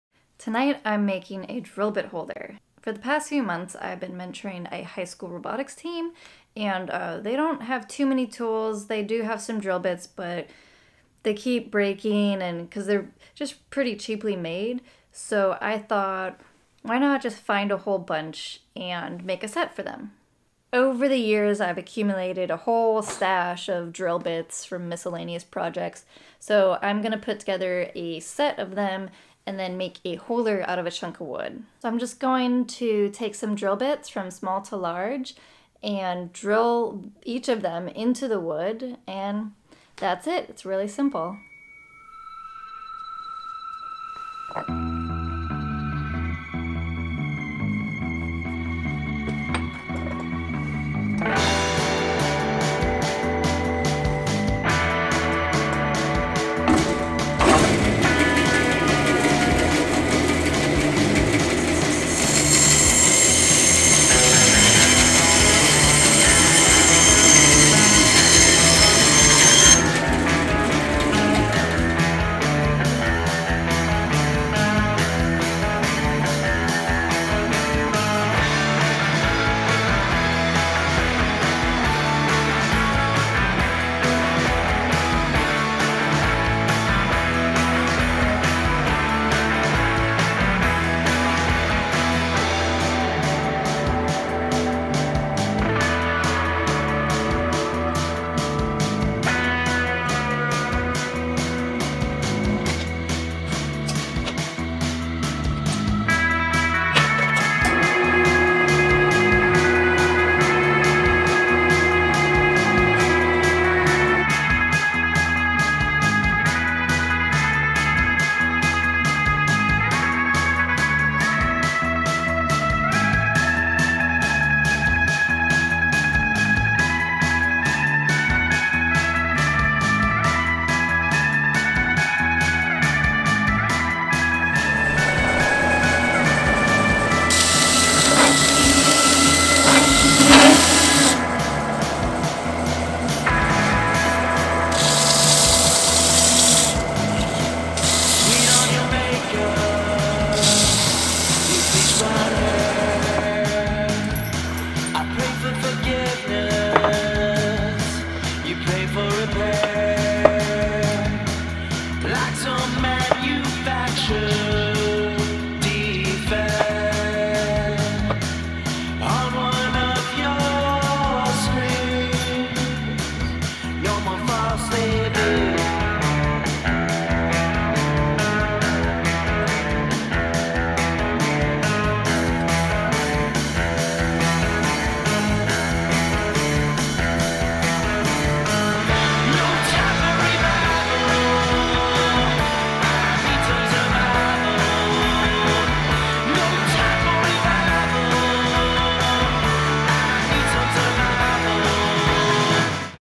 I Made A Drill Bit Holder
I made a simple drill bit holder out of a scrap of wood. Fast, easy, and very few tools needed.